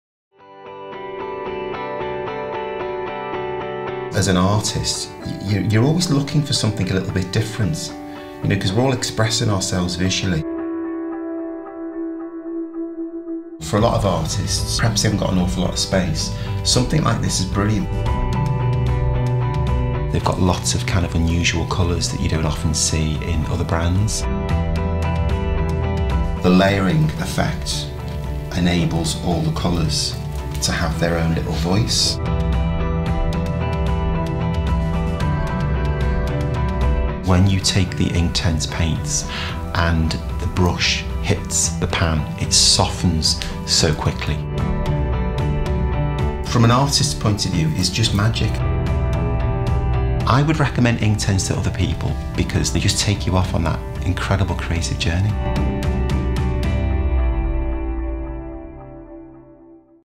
Derwent Inktense Paint Pan Set - Official Promo Video (60 seconds)
The Derwent Inktense Paint Pan Travel Set contains the unique Inktense formulation found in our pencil and block ranges. Unlike traditional watercolour, washes of vivid paint can be applied without dissolving previously dried layers.

Ideal for travel with everything needed to paint on the go, this set contains 12 highly lightfast Inktense paint pans, a mini waterbrush, a sponge and five mixing palettes. Inktense is suitable for use on paper and fabric for a wide range of fine art and craft projects